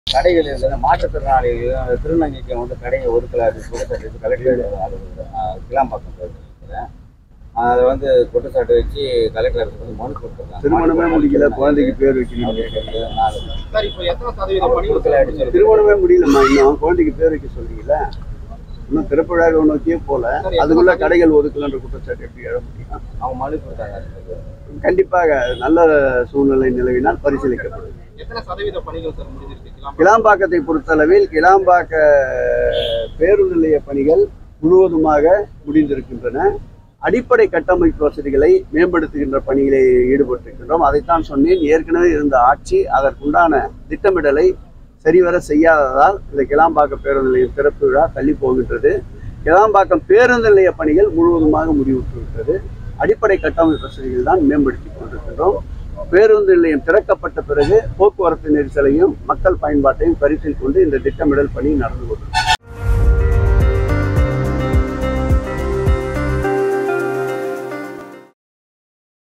திருமணமே முடில குழந்தைக்கு எப்படி பெயர் வைக்குறது.. MinisterSekarbabu | KilambakkamBusStand
திருமணமே முடில குழந்தைக்கு எப்படி பெயர் வைக்குறது.. கிளாம்பாக்கத்தில் புதிய பேருந்து நிலைய கடைகள் குறித்த கேள்விக்கு அமைச்சர் சேகர்பாபு பதில்..!